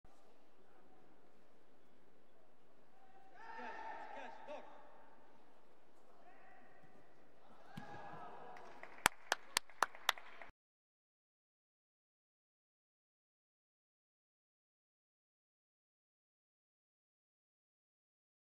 Lee's Sleeve Seoi
Lee Harim does an end-sleeve drop seoi which is vogue these days. Look at how low he drops. Uke is spun right over.

(Video clip courtesy of IJF).